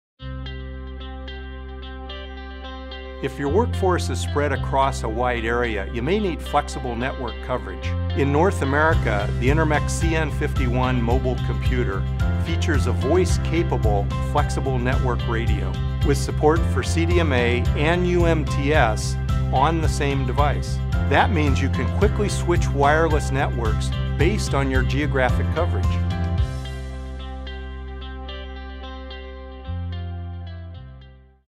Barcode CN51 Mobile Computer_ Wireless Coverage
เครื่องอ่านบาร์โค้ด ชนิดพกพาCN51 Mobile Computer Versatility speaks for itself. The Intermec CN51 offers thousands of ways to get more done with a choice of Android or Windows, a large, resistive multi-touch display, Flexible Network™ Radio (in North America), superior scanning, processing, battery power and more.
O/S VERSATILITY Supports Microsoft® Windows Embedded Handheld 6.5 and Android™ 4.1 Operating Systems for flexible application support and migration*
DISPL AY Large 4" brilliant WVGA multi-touch display allows for more graphical user interface, display-centric applications
RUGGED Withstands multiple 1.5m (5ft) drops to concrete across the operating temperature range and an IP64 seal rating against rain and dust
SCANNING Features best-in-class motion tolerance, and support for omni-directional 1D & 2D barcode scanning
FLEXIBLE NETWORK Features WAN radio options with voice and data support including UMTS and Flexible Network UMTS/CDMA, PTT, as well as 802.11 a/b/g/n, Bluetooth, and GPS with LONASS support
BATTERY Industry-leading performance that lasts a full shift and beyond, enhanced by smart battery technology
PROCESSOR Equipped with a power-efficient 1.5GHz dual core multi-engine Texas Instruments OMAP4 processor architecture, with 1GB RAM and 16GB Flash memory